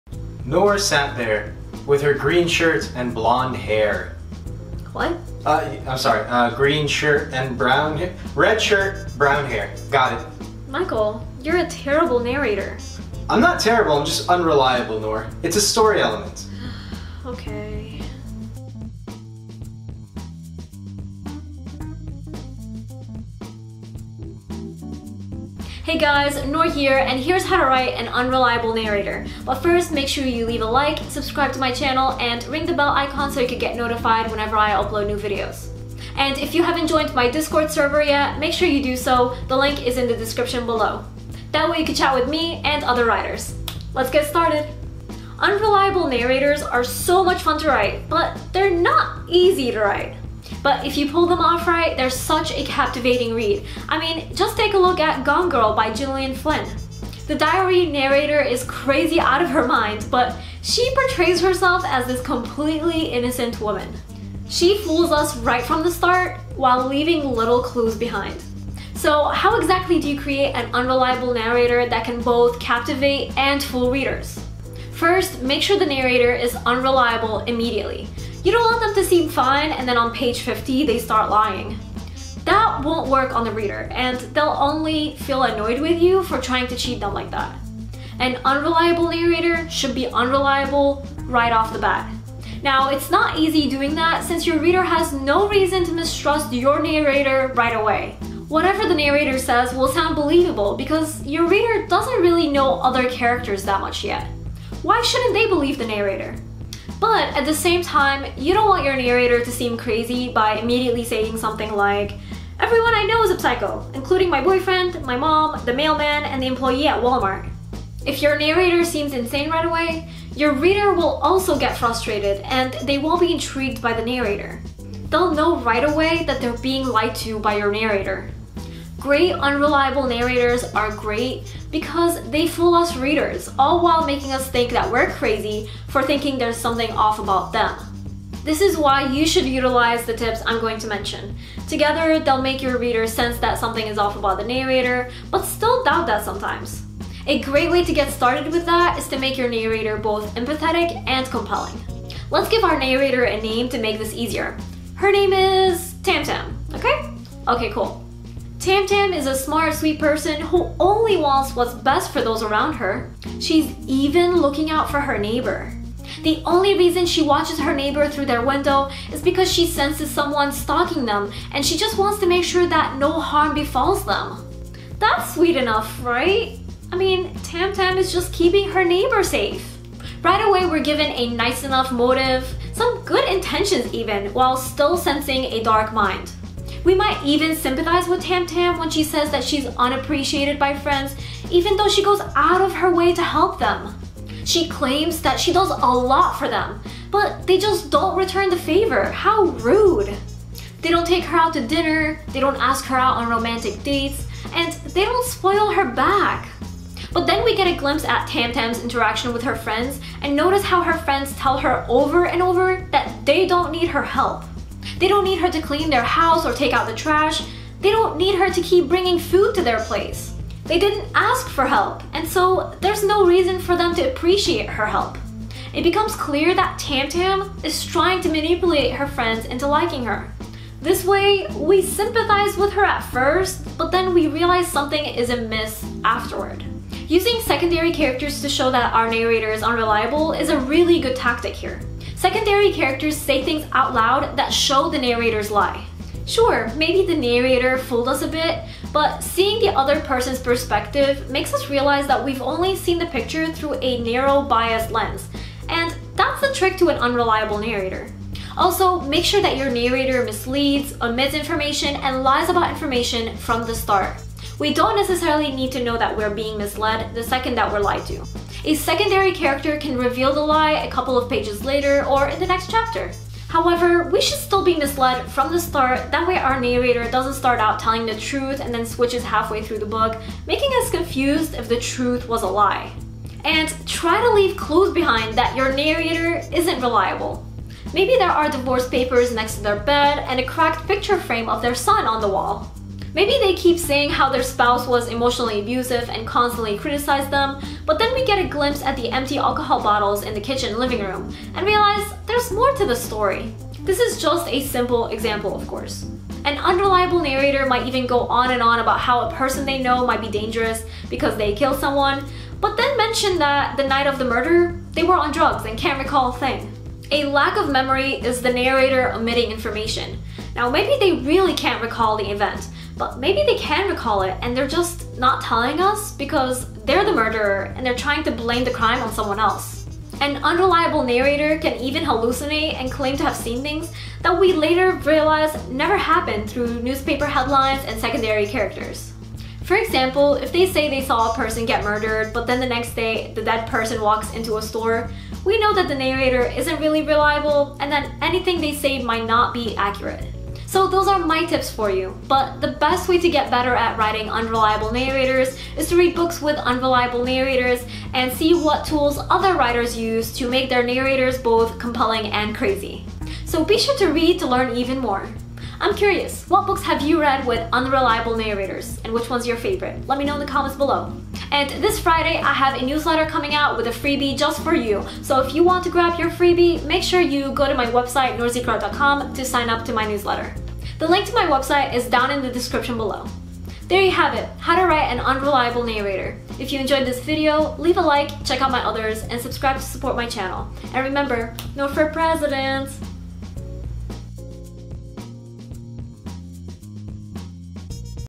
How To Write An Unreliable Narrator | Writing Advice
An unreliable narrator can be a fun way to compel readers while throwing them on a roller coaster ride. So in this writing advice video, I'm showing you how to write an unreliable narrator.